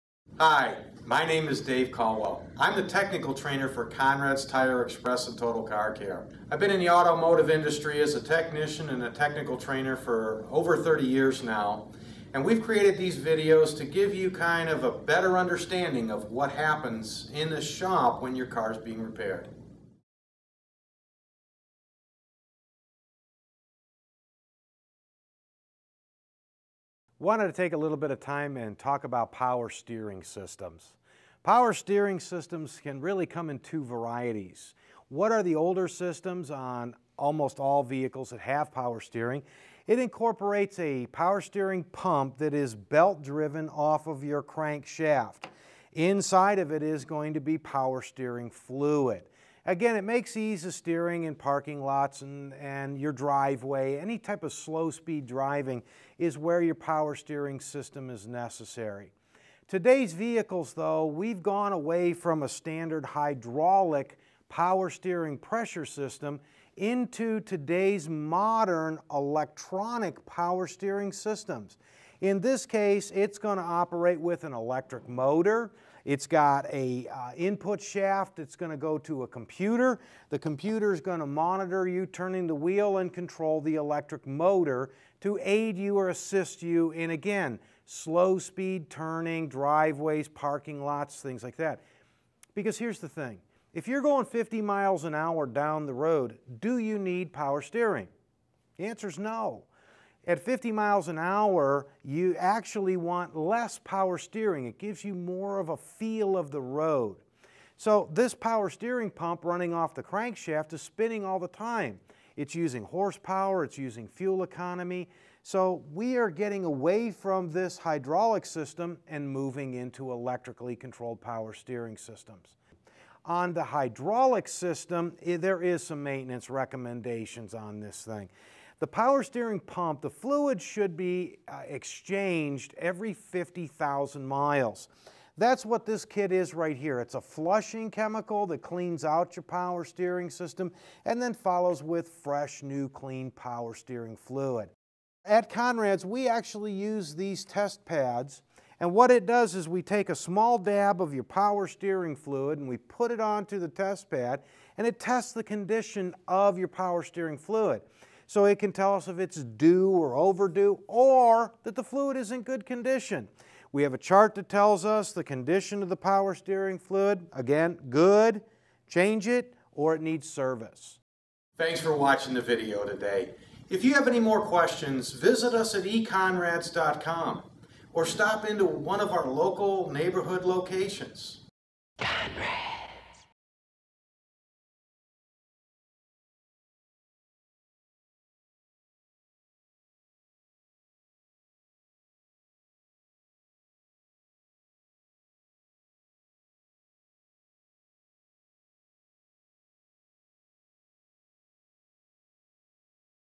Power steering: How power steering helps you drive your vehicle.
Learn about how power steering or power-assisted steering assists in the operation of you vehicle.